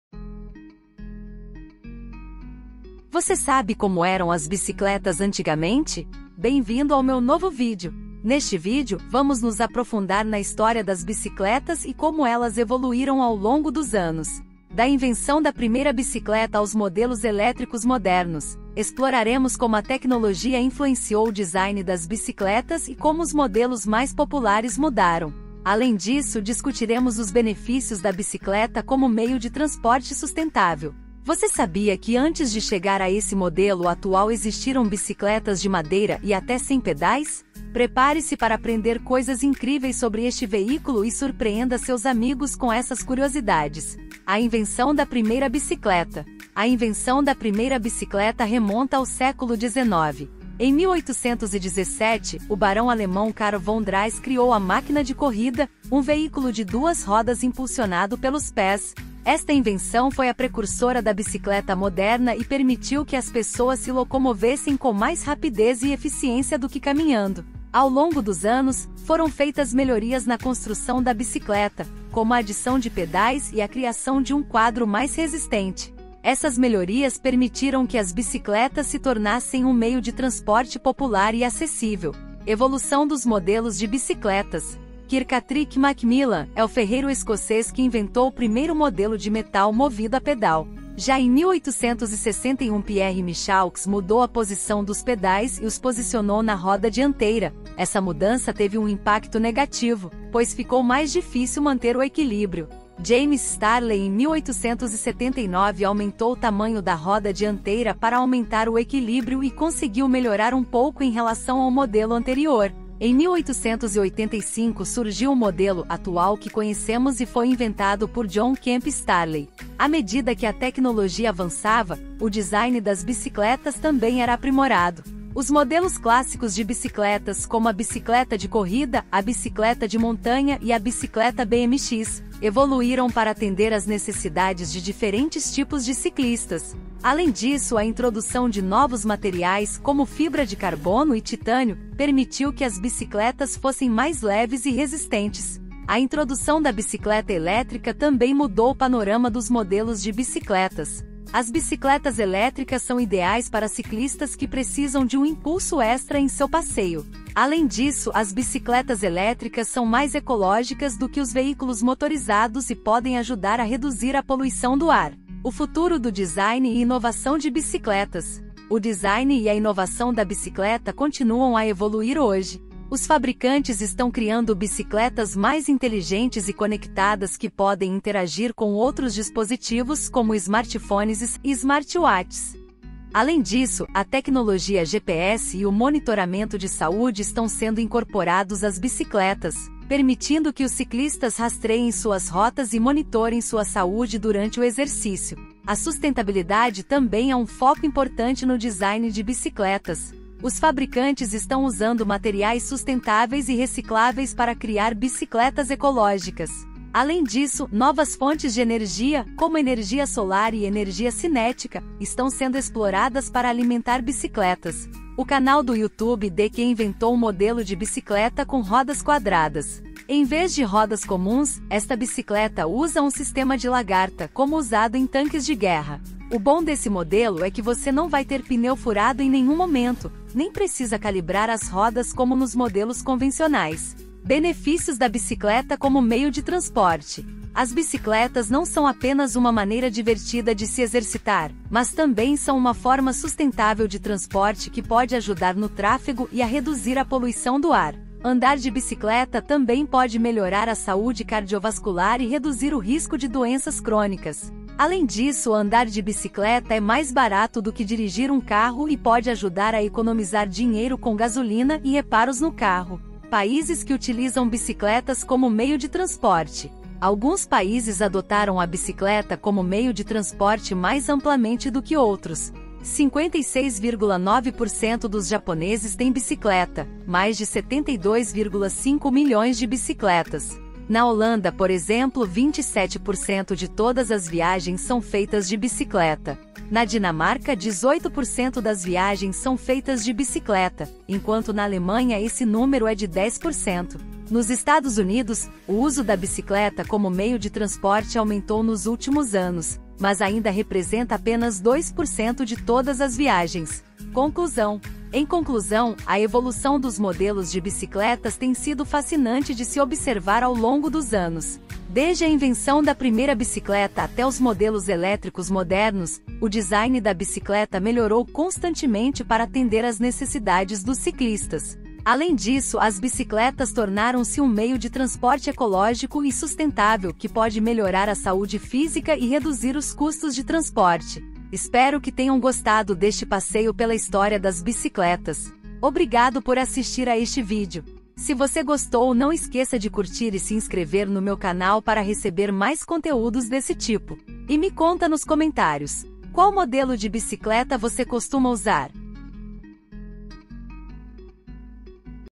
🚴 Você sabe como eram as bicicletas antigamente?
Bem-vindo ao meu novo vídeo! Neste vídeo, vamos nos aprofundar na história das bicicletas e como elas evoluíram ao longo dos anos. Da invenção da primeira bicicleta aos modelos elétricos modernos, exploraremos como a tecnologia influenciou o design das bicicletas e como os modelos mais populares mudaram. Além disso, discutiremos os benefícios da bicicleta como meio de transporte sustentável. Você sabia que antes de chegar a esse modelo atual existiram bicicletas de madeira e até sem pedais? Prepare-se para aprender coisas incríveis sobre este veículo e surpreenda seus amigos com essas curiosidades.